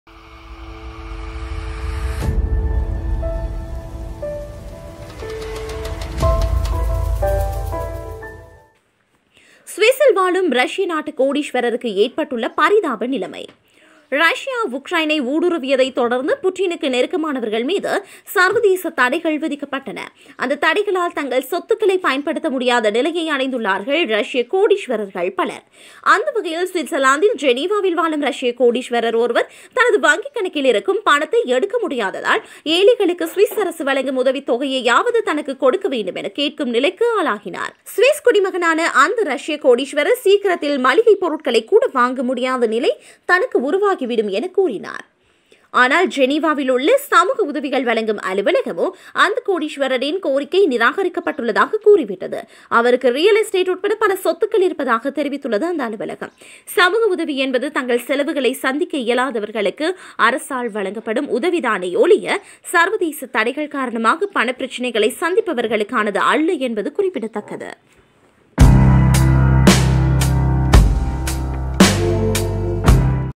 சுவிஸில் வாழும் ரஷ்ய நாட்டு கோடீஸ்வரருக்கு ஏற்பட்டுள்ள பரிதாப நிலைமை
ரஷ்யா உக்ரைனை ஊடுருவியதைத் தொடர்ந்து, புடினுக்கு நெருக்கமானவர்கள் மீது சர்வதேசத் தடைகள் விதிக்கப்பட்டன.